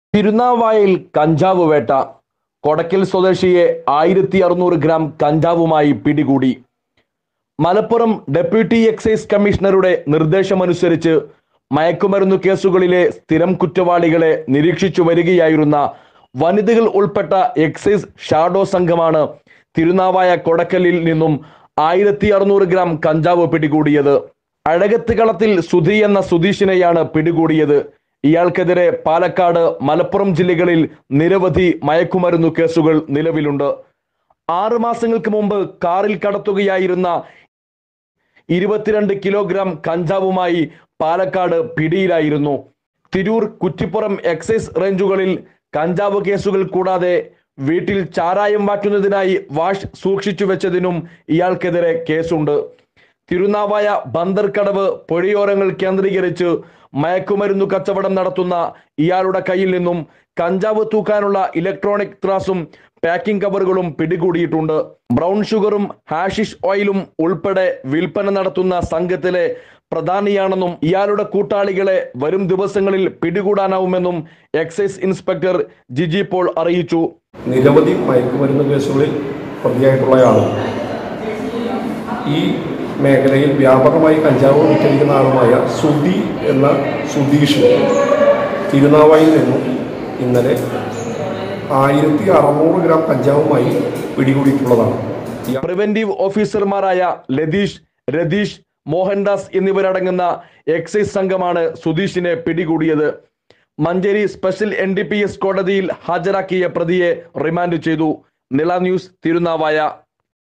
man arrested കൊടക്കൽ സ്വദേശി സുധീഷിനെ 1600gm കഞ്ചാവുമായി പിടികൂടി
Subscaibe nila news Chanel
Malayalam news